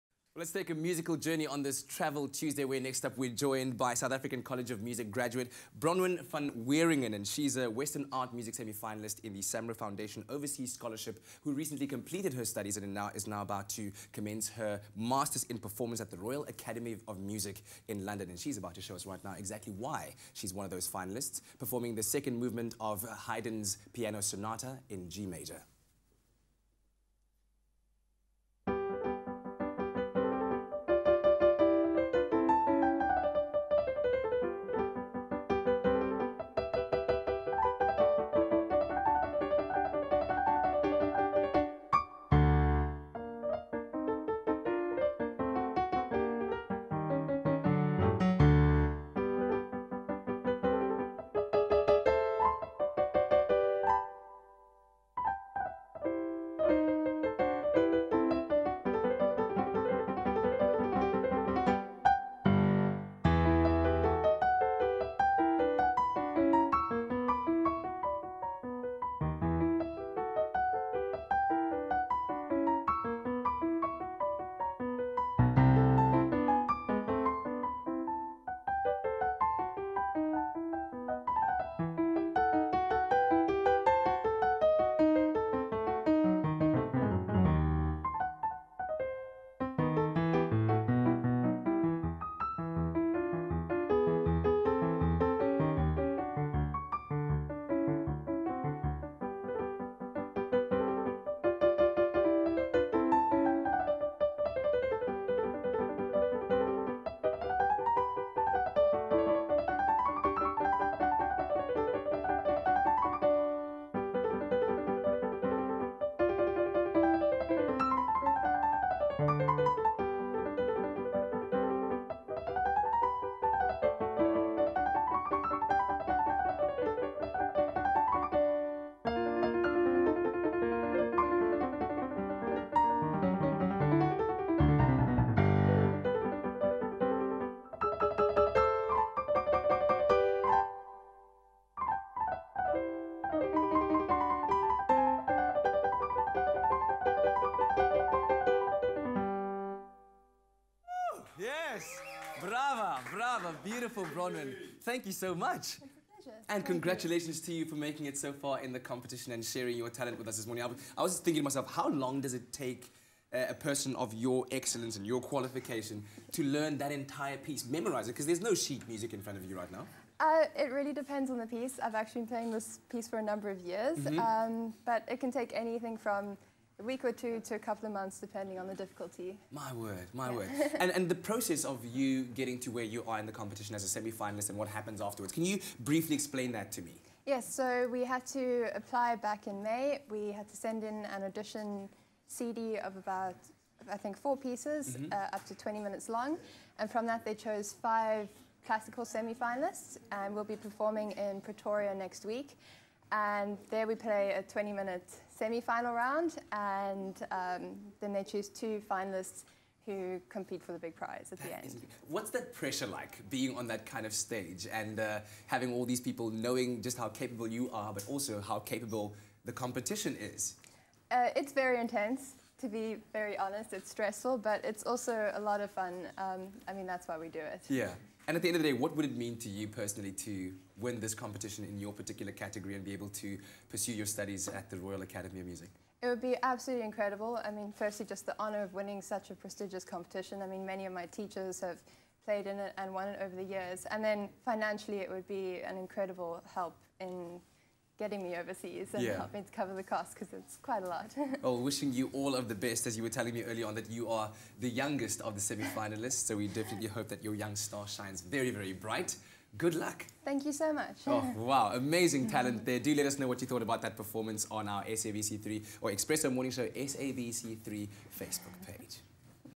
Bronwyn van Wieringen (SAMRO Semi-Finalist 2/3) performs
The SAMRO Foundation unveiled the semi-finalists who have been selected to compete for one of the most sought-after music education awards in Jazz and Western Art music.
After testing their skills and endurance during another two intensive rounds of competition, the two winners will each receive a R200 000 scholarship to pursue their professional development internationally.